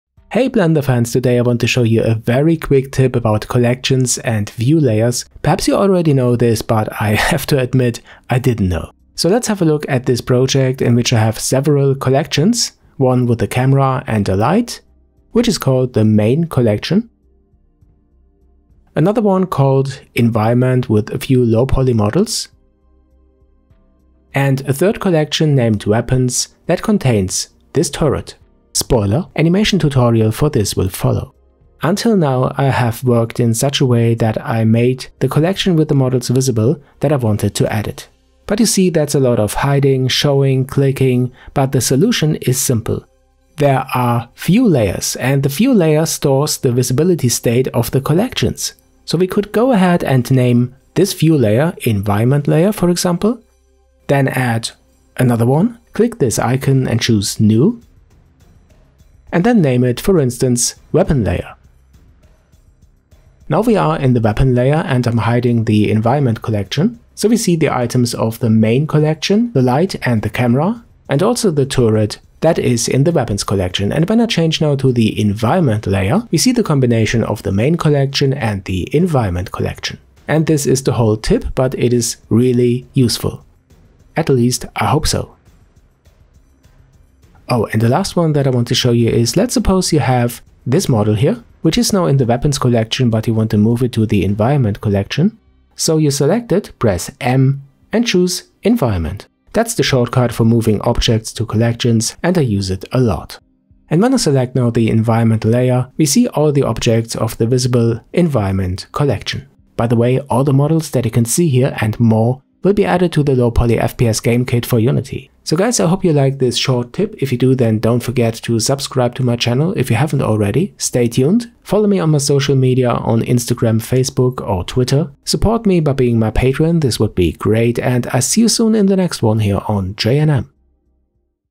Blender 2.8 Quick Tip : Collections & Layers
In this Blender video quick tip I explain how to use Collections in combination with View Layers which store the visibility states for collections.